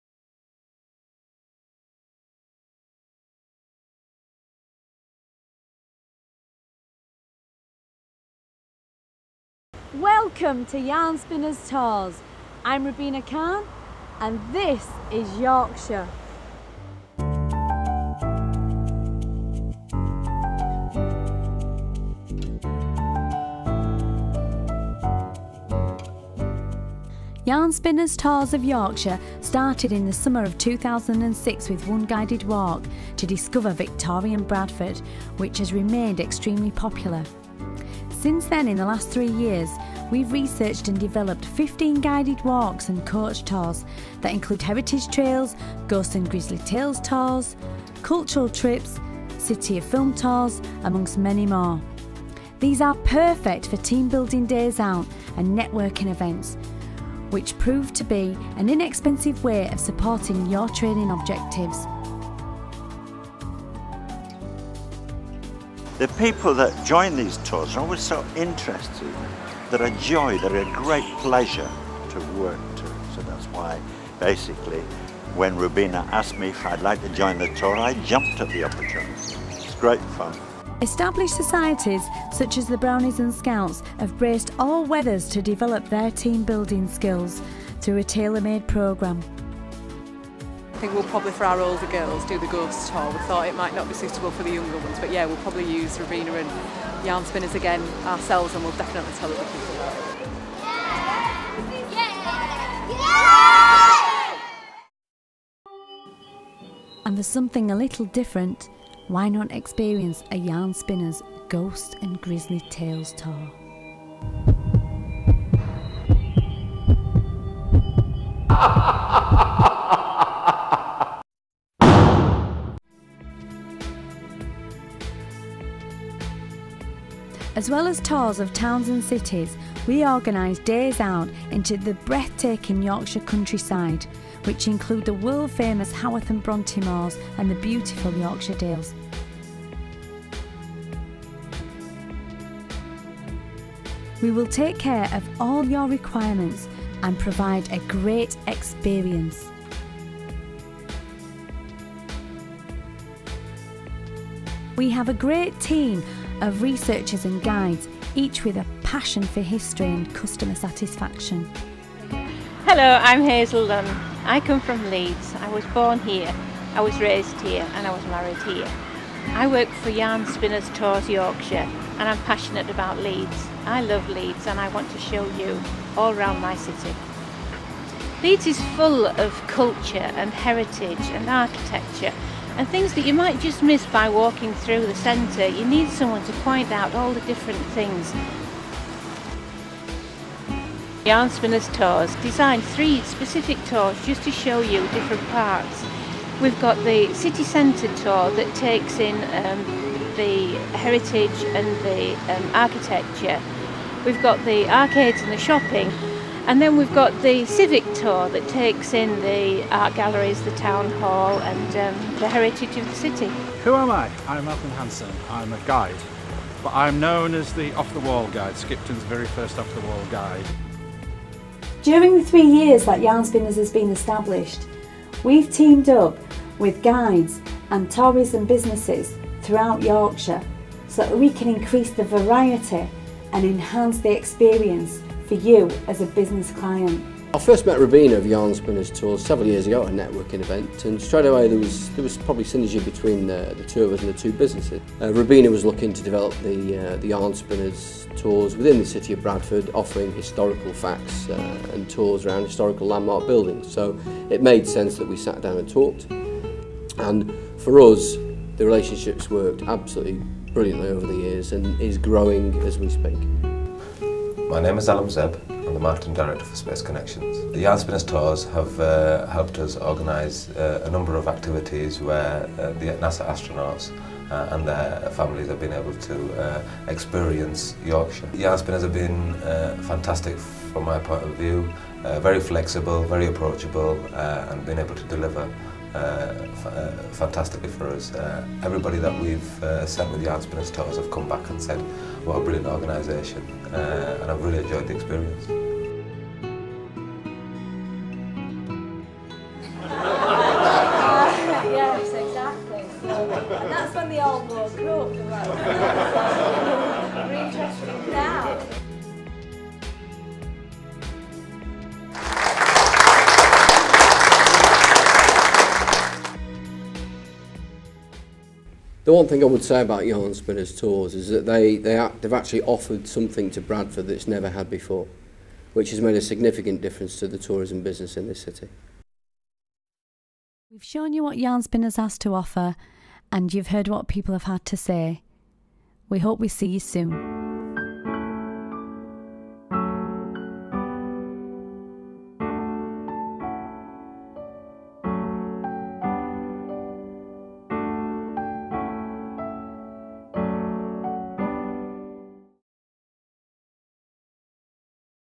Yarn Spinners Tours 2009- JAR Film Productions
We took Six weeks to film and Produce this video for YarnSpinners, this involved on location filming as well as interviews, we then produced the final edit adding the music (all copywrited) and recording voice over for the whole video.